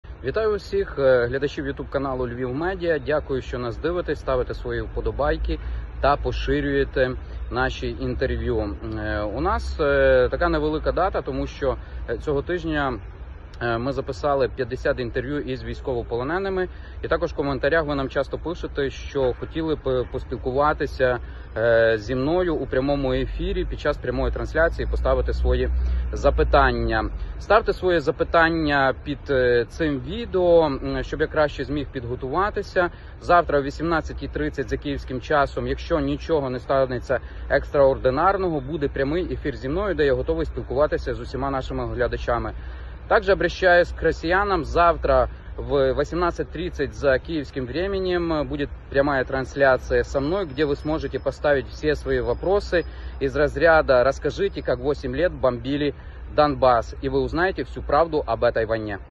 Перший стрім! Любомир Ференс звернувся до глядачів
Вже завтра, 1 грудня, Любомир Ференс о 18:30 проведе перший стрім. Звичайно, якщо ніхто не зіпсує наші плани(( Чекаємо ваші питання у коментарях.